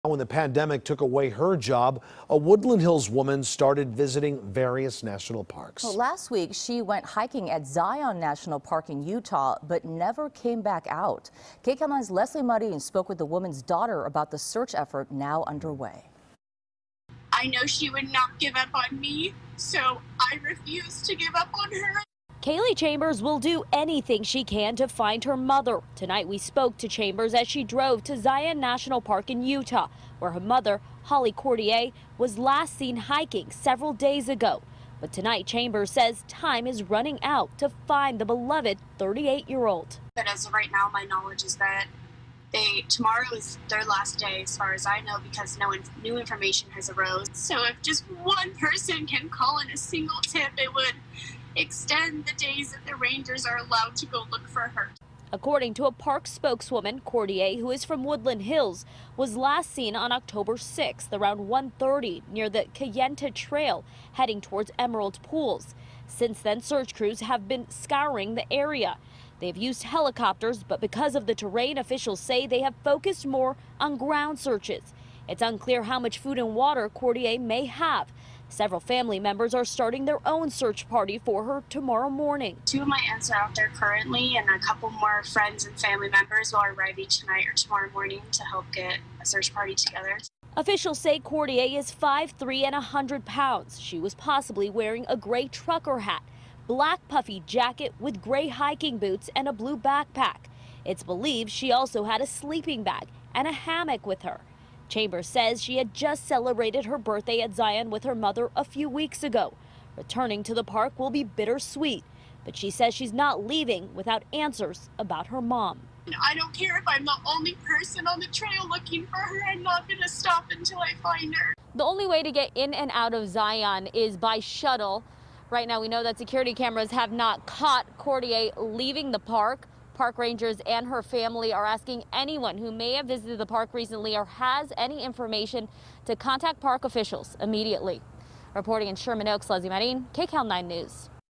Search Continues For Woodland Hills Woman Who Went Missing In Utah's Zion National Park
Holly Suzanne Courtier, 38, was last seen at about 1:30 p.m. on Oct. 6. The National Park Service, which is searching for Courtier with the Washington County’s Sheriff’s Office, says her intended travel plan was not known.